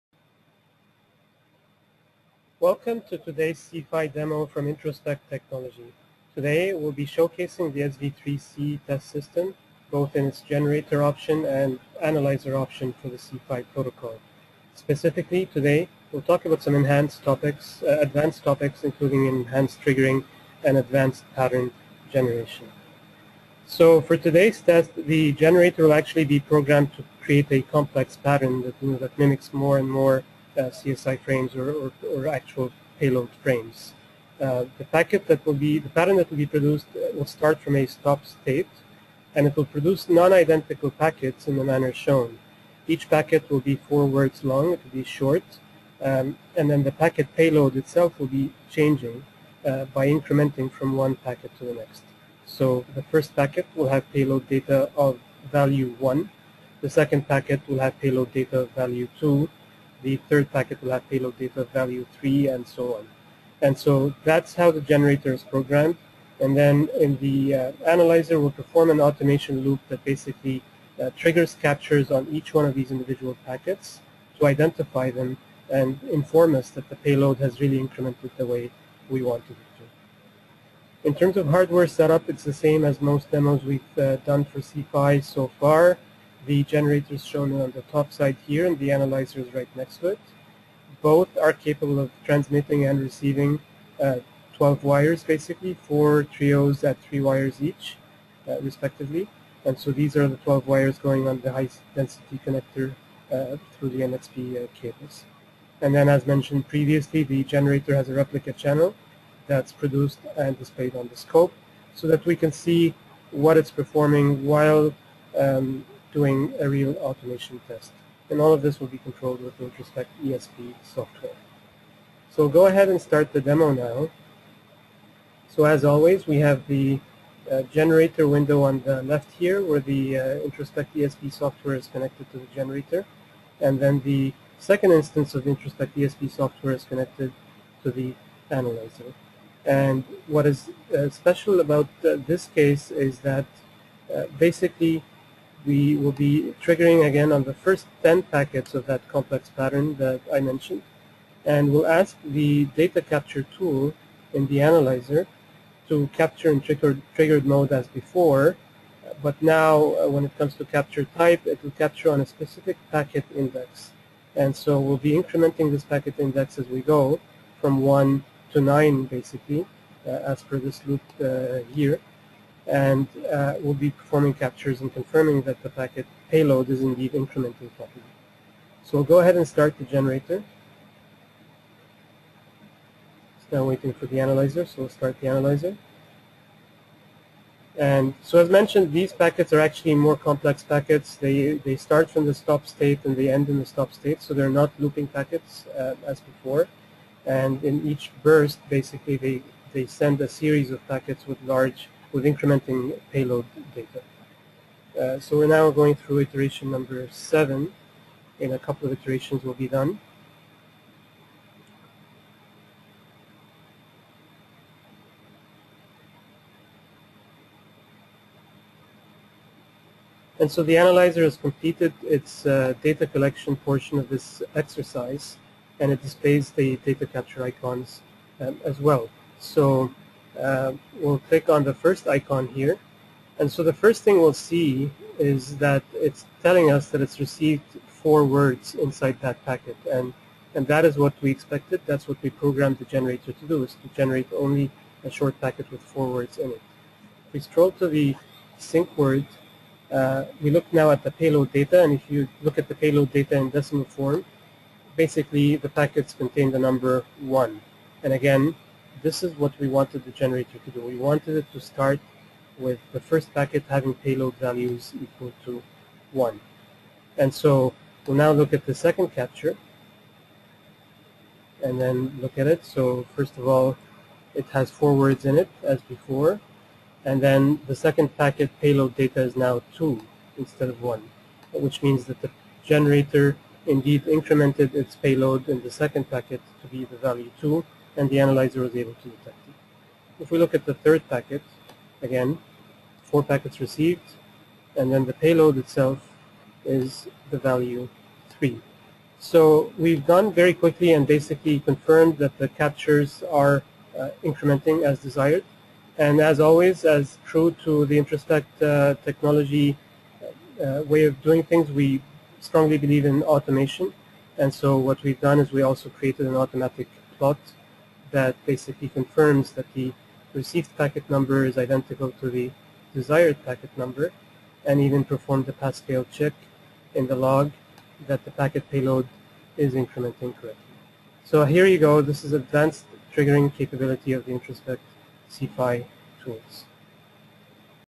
Advanced Topics in C-PHY
More advanced testing is performed in this video. A non-repeating packet is programmed using the generator and then measured with the analyzer. True to Introspect's tradition, a characterization loop is performed with autonomous pass-fail detection.